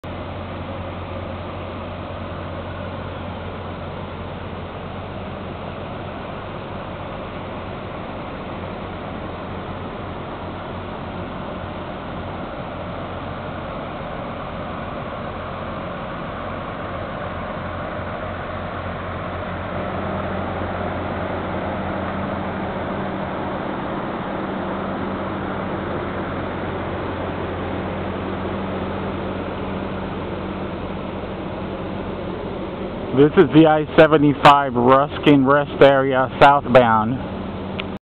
Florida Vandweller I-75 Hillsborough County - Interstate Rest Area Southbound
Visit home page for Tshirts and donations Huge space here for trucks and RV plus cars, just south of the Sun City Center exit and a few miles north of the I-275 junction and the Manatee County border. Safe nighttime security and janitor always there